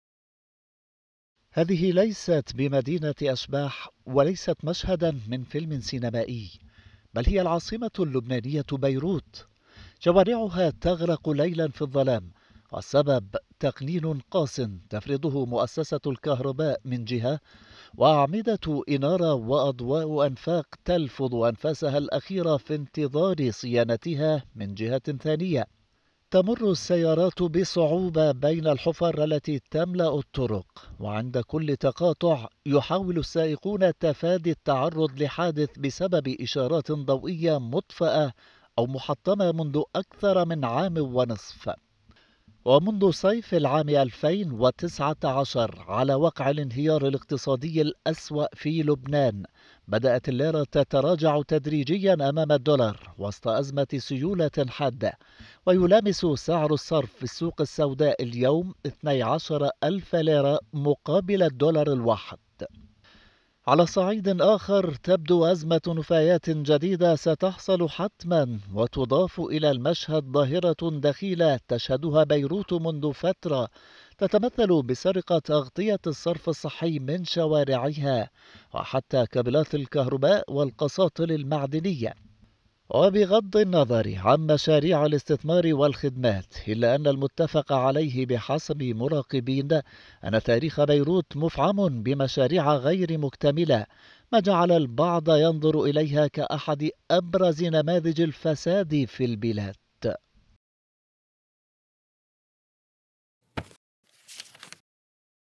شاهد كيف تحولت بيروت الى مدينة أشباح | تقرير
لاذقية كعنان، العاصمة اللبنانية بيروت، المصائب تتوارد عليها تباعاً، أزمة سياسية طال أمدها، وواقع اقتصادي على شفا الانهيار، وبنى تحتية متهالكة وسط امتعاض الأهالي الذين ضاقوا ذرعاً بالأزمات التي تحط على رؤوسهم.. التقرير التالي يسلط الضوء على جزء من المأساة.